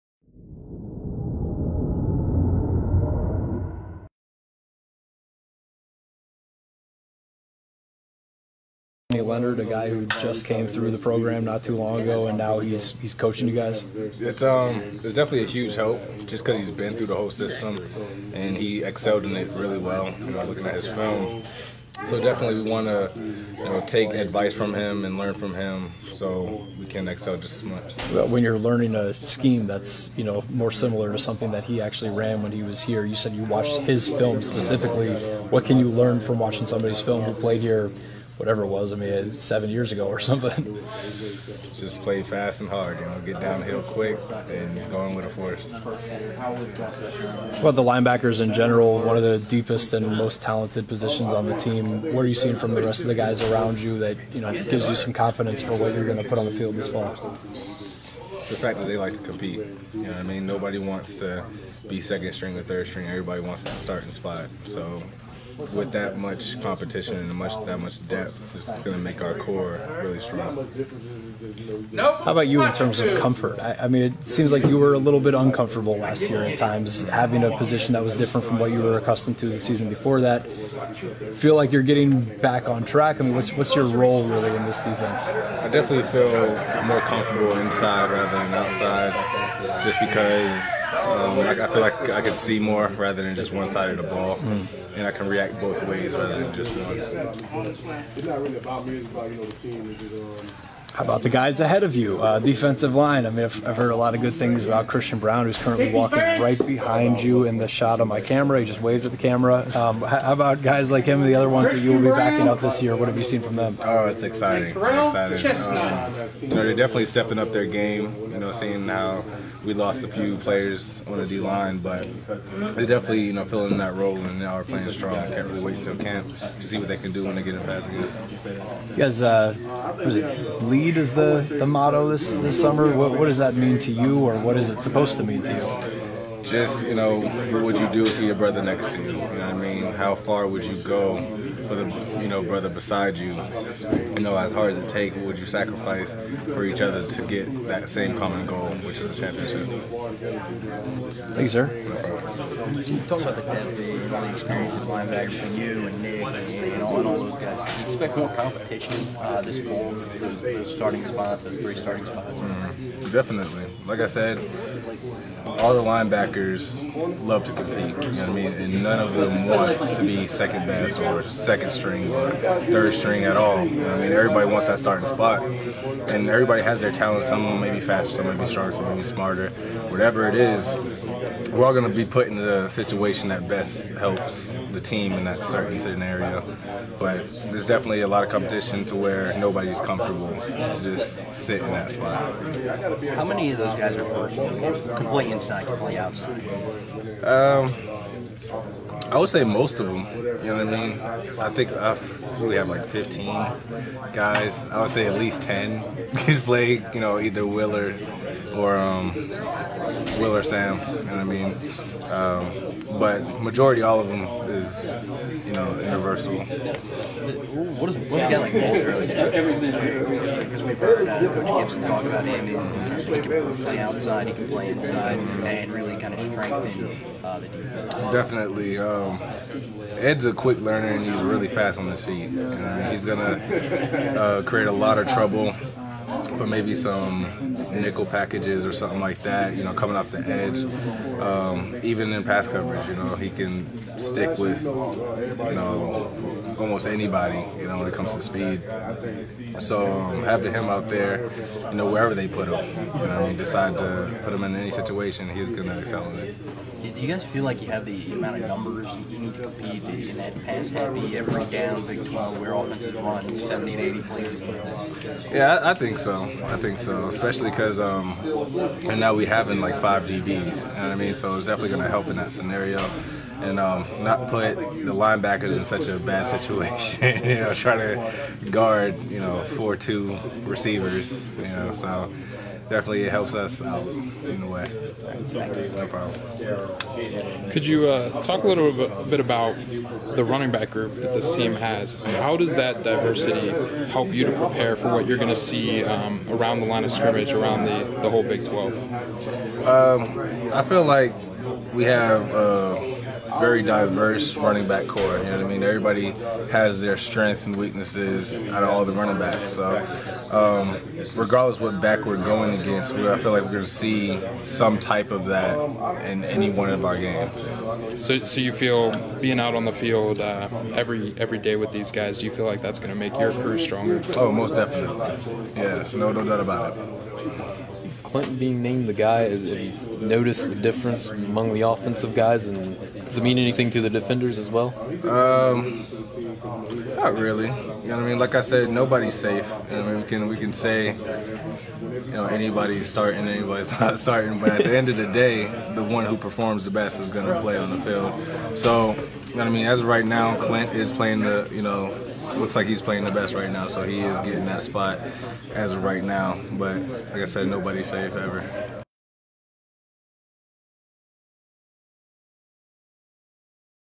WVU Football: Isaiah Bruce 6-26-14
WVU football player Isaiah Bruce talks to the media before fall camp.